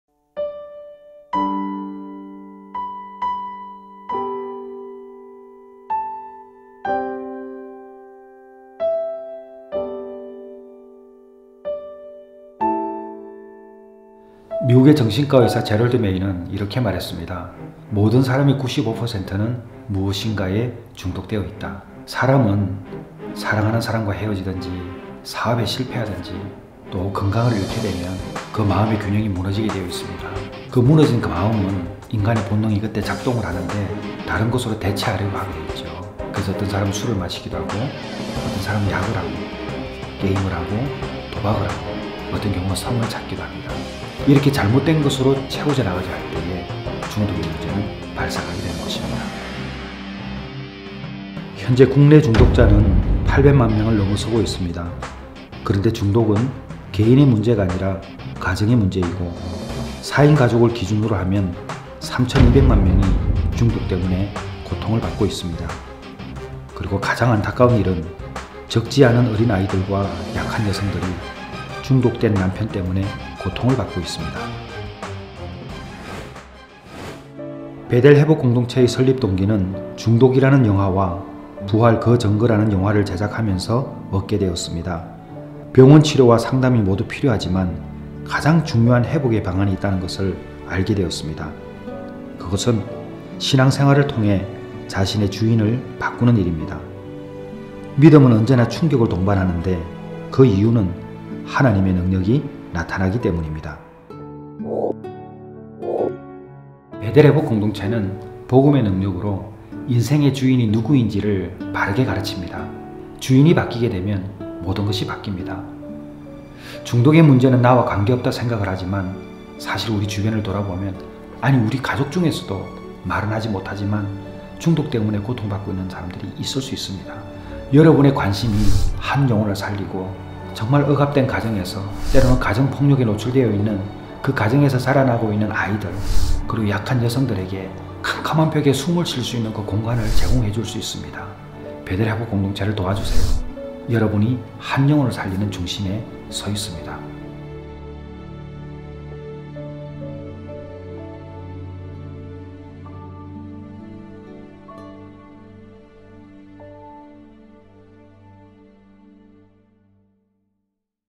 베델회복공동체 소개 영상
베델회복공동체 (파이오니아21연구소)는 중독자들을 위한 무료 상담과 영화치유 프로그램을 통해 회복을 돕고 있습니다. 중독된 사람들은 병원의 치료와 상담이 필요하며 신앙을 가짐으로 인생의 주인이 누구인지 분명히 알아야 합니다. 베델회복공동체는 기독교의 복음이 유일한 회복의 길임을 선언하고 사역하는 기관입니다.
[중독 및 상담 사역을 후원하고 싶습니다]
베델회복공동체 / 농협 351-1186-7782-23
우리은행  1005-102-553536 내가사랑하는교회